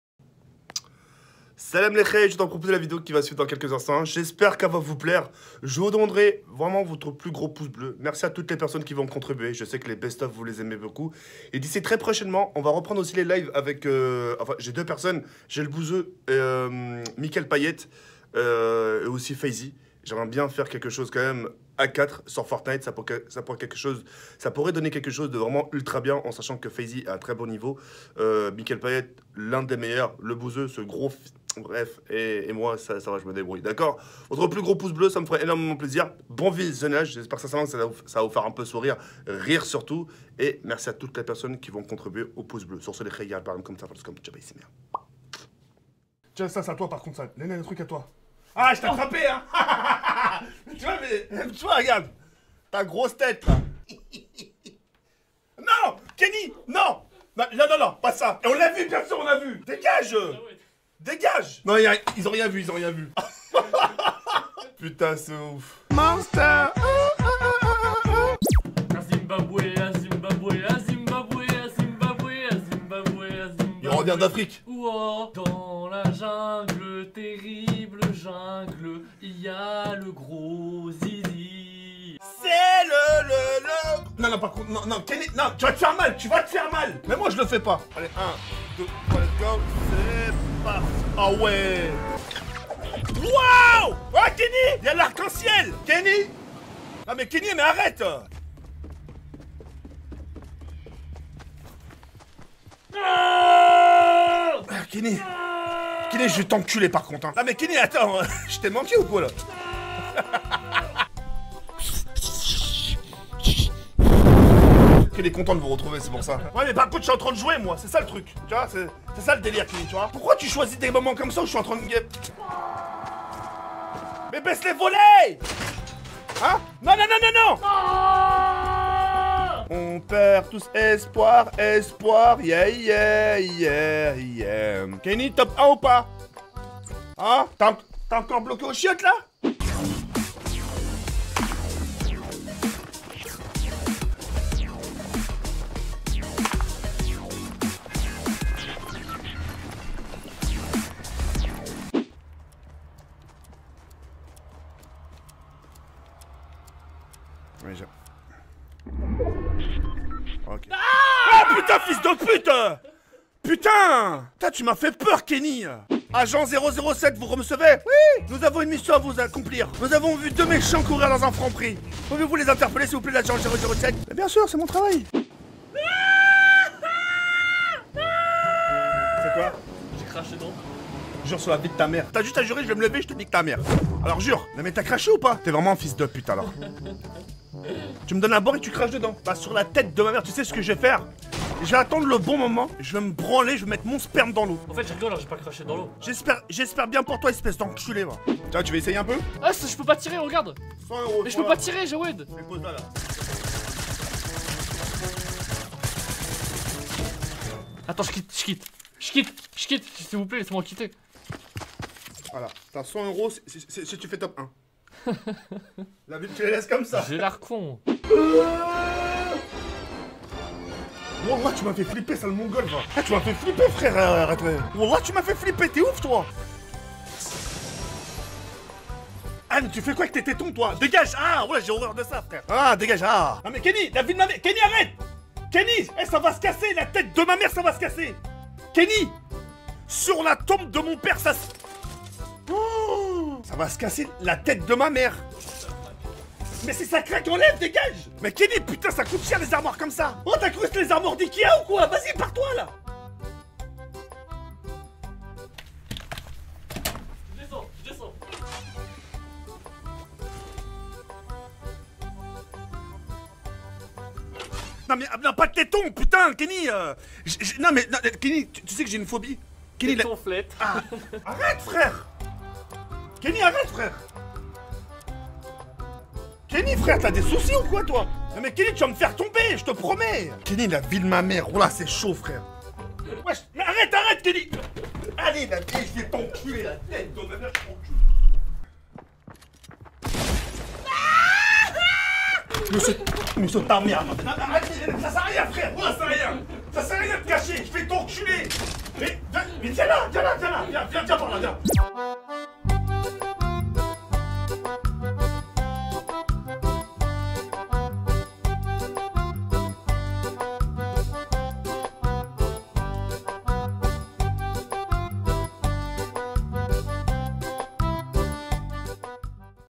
KENNY MONTRE CES FESSE EN PLEIN LIVE !
Omg il montre son cul en plein live devant 10 000 personnes !

Pendant le live avec Thekairi78